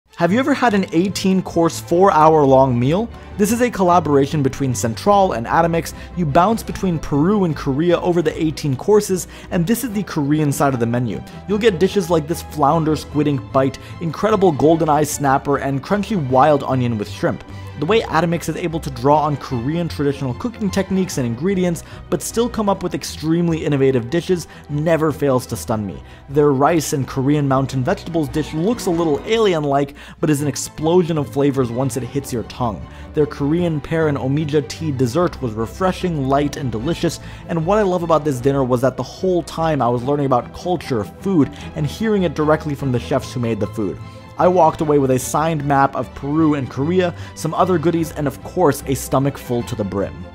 What Happens When Two of the World's Best Restaurants Meet
Pt. 2 of the best meal of my life.